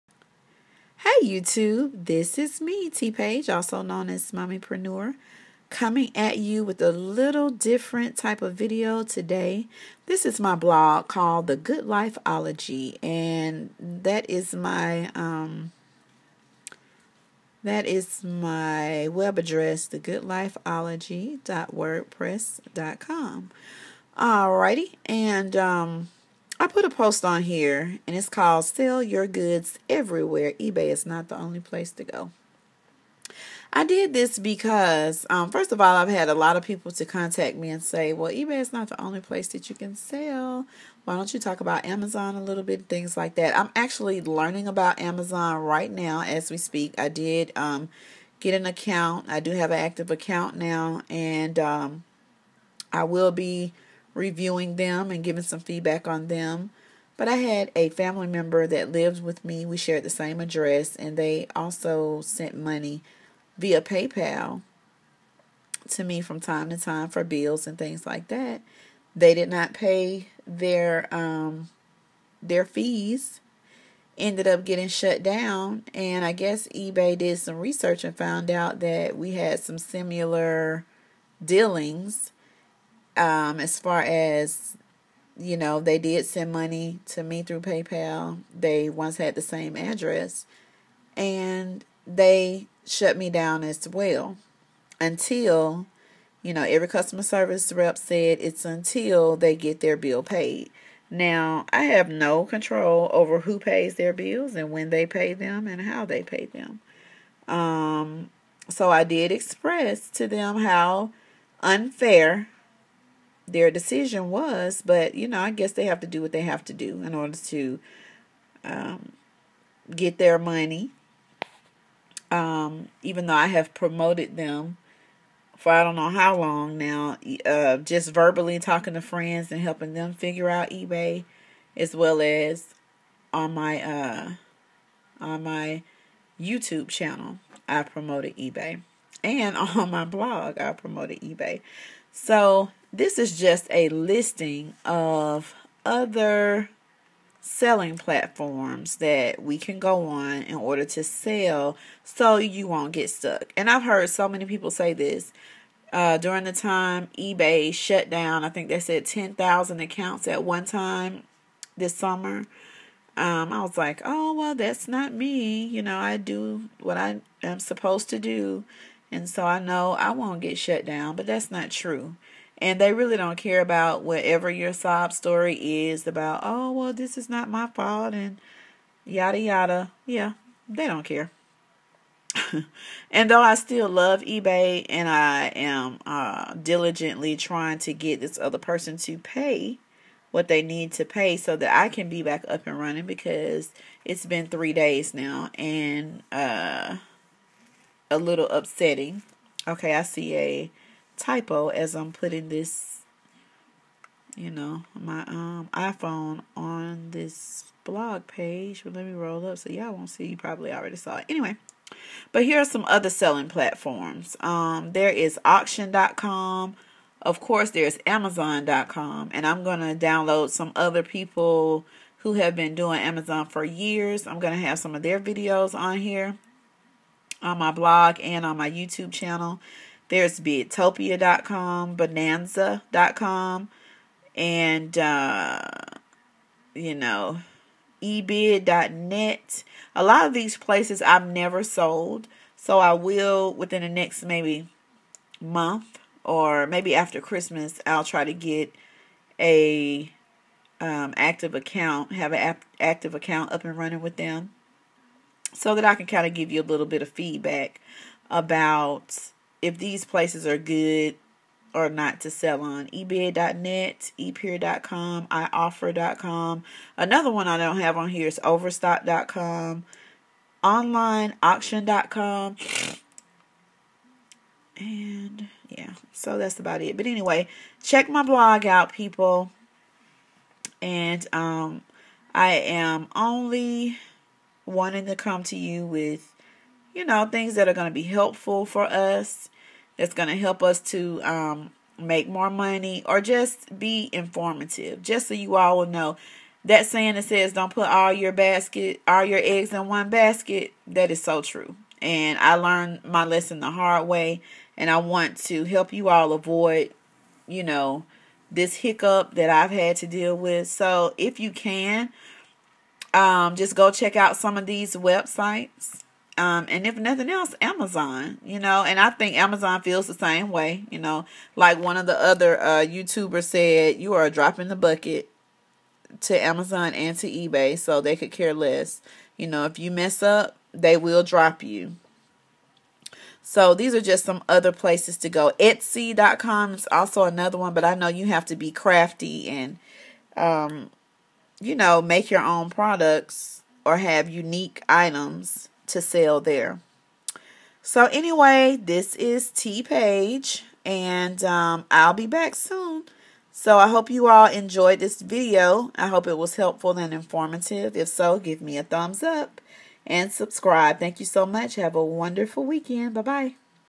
Don't get stuck on just selling on EBay!! There are other places to Sell and Succeed!!!
Find out why EBay has me temporarily restricted. Go to my blog for a list of other selling sites. Also, of course, look at:
Amazon.com
Overstock.com
Etsy.com